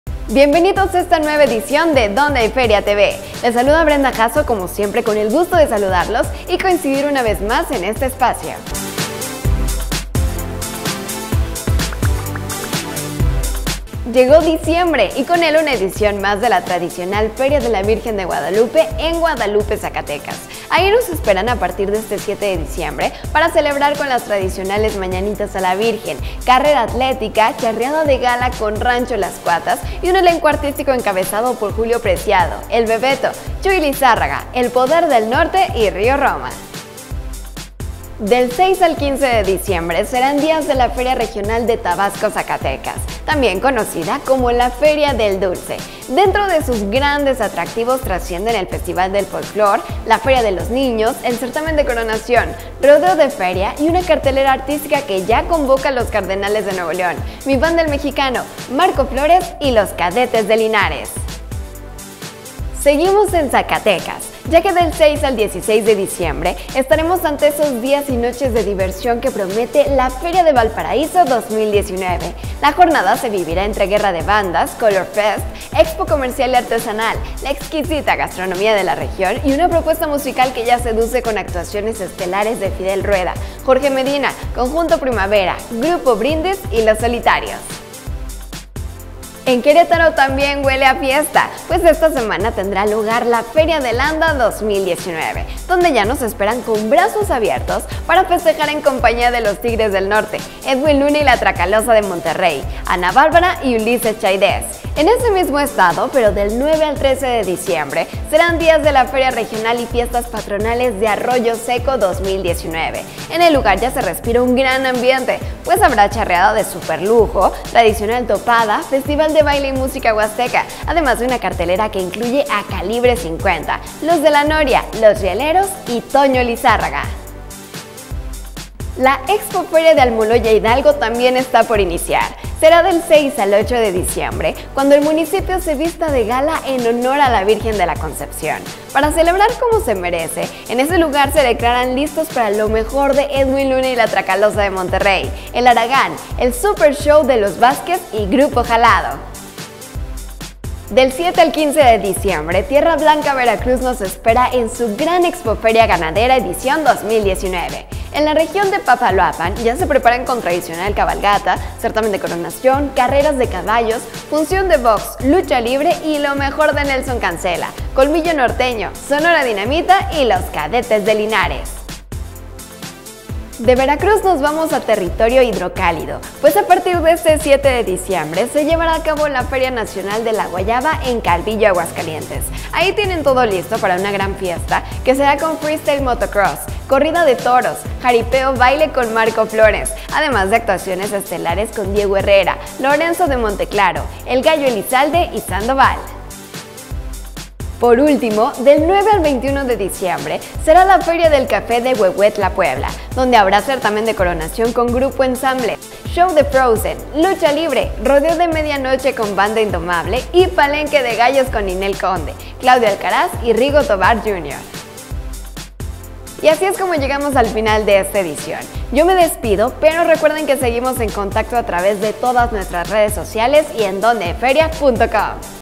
Ferias de Guadalupe, Tabasco, Valparaíso, Landa, Arroyo Seco, Almoloya, Tierra Blanca, Calvillo...
🎉¡QUE COMIENCEN LAS FIESTAS DECEMBRINAS!🤩

No te pierdas DóndeHayFeriaTV📺 porque hoy te contamos de

✅ Feria de la Virgen de Guadalupe, Feria Regional Tabasco y Feria Valparaíso ZAC🎡
✅Feria Landa y Feria ArroyoSeco QRO🎢
✅Feria Almoloya HGO⛪️
✅Feria Ganadera TierraBlanca VER🐂
✅Feria Nacional de la Guayaba Calvillo AGS😋
✅Feria del Café Huehuetla PUE☕️